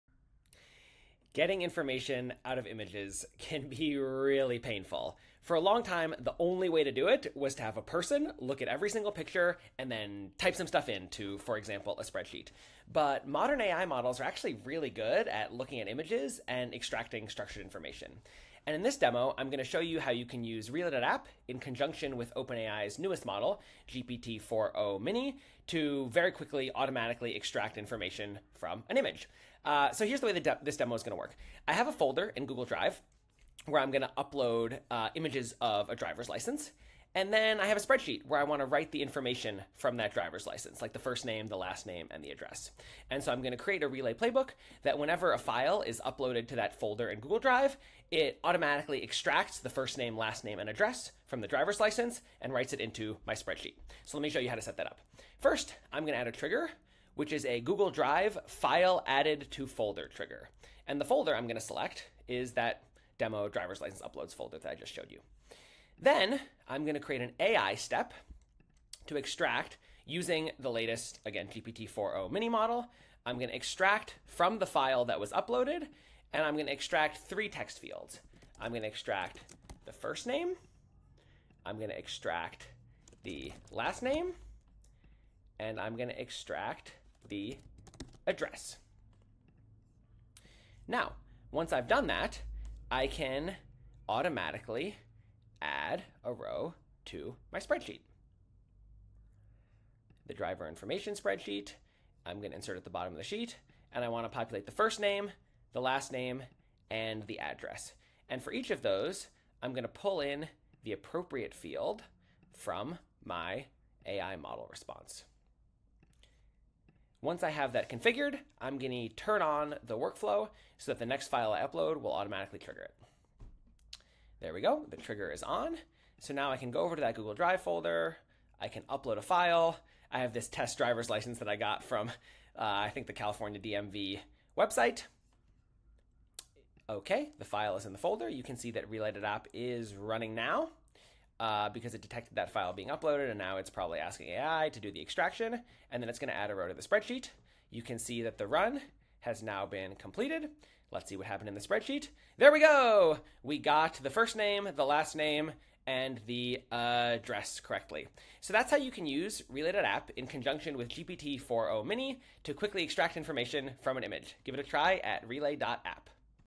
How to Use GPT-4o mini to Extract Information from an Image
If you've ever had to extract information from an image and type it into a spreadsheet, you know how tedious and painful that can be. Luckily, modern AI models - including OpenAI's most recent model GPT-4o mini - are really good at this. In this demo, I'll show you how to automatically extract information from a new image that's uploaded to Google Drive and write it into a Google Sheet.

Chapters
0:00 - Introducing the use case
0:55 - Setting up the 'file added to folder' trigger
1:05 - Add the 'AI Extract' step using GPT-4o mini
1:31 - Add the 'add row to spreadsheet' step
1:56 - Turn on and test the workflow